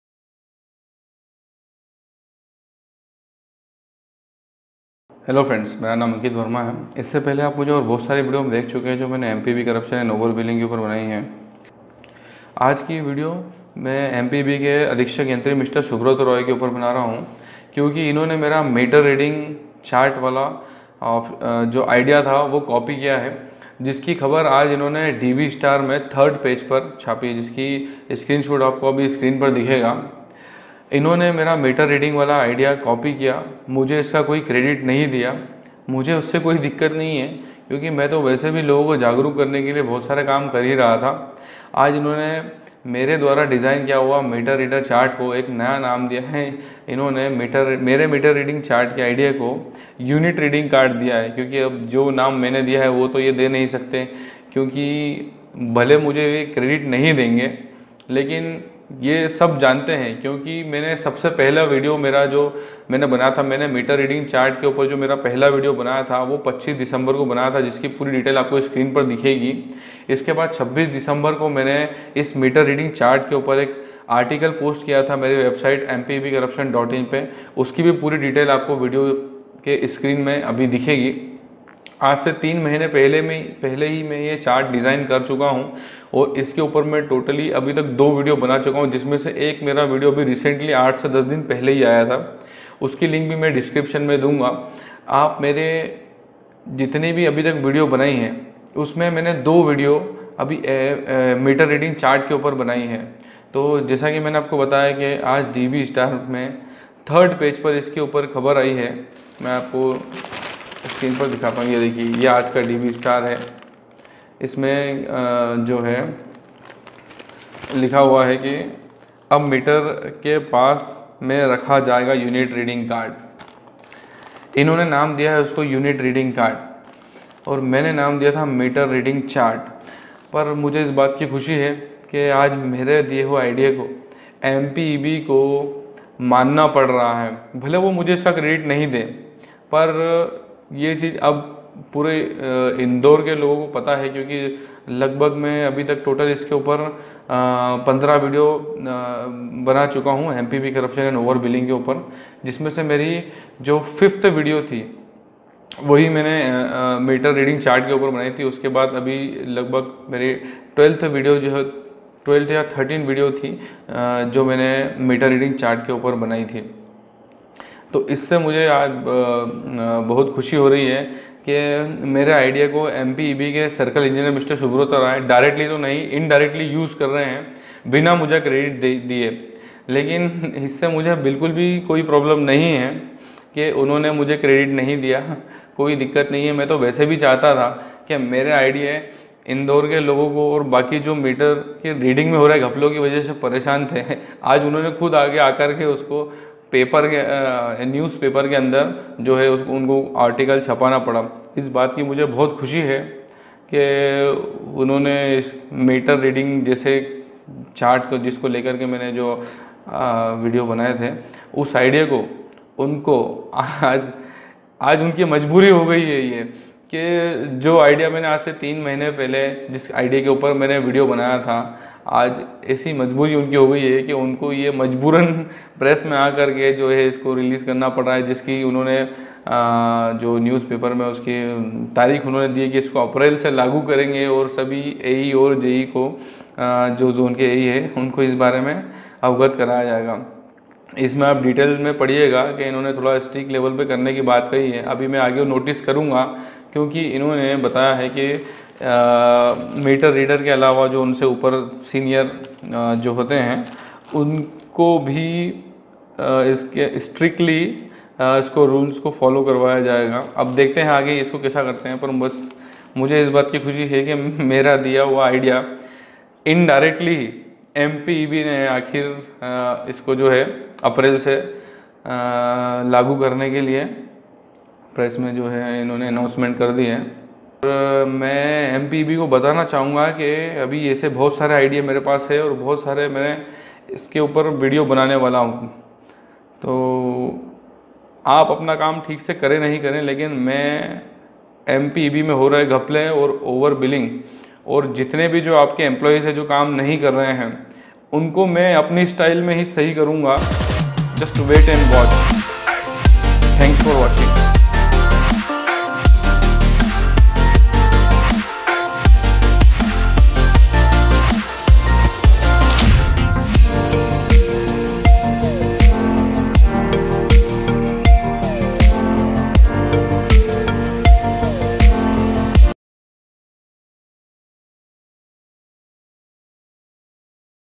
इंदौर बिजली विभाग के अधीक्षक यंत्री सुब्रतो रॉय ने मेरा “मीटर रीडिंग चार्ट” का आईडिया चुराया
Indore MPEB Circle Engineer Copy My "Meter Reading Chart,
MPEB के अधीक्षक यंत्री Mr.सुब्रत रॉय ने मेरा “मीटर रीडिंग चार्ट” का आईडिया चुराया,

बिजली विभाग से संबंधित किसी समस्या के लिए हमसे फ़ोन पर बात कर परामर्श चाहते है तो Call Consultation बुक कीजिये

My Tools :-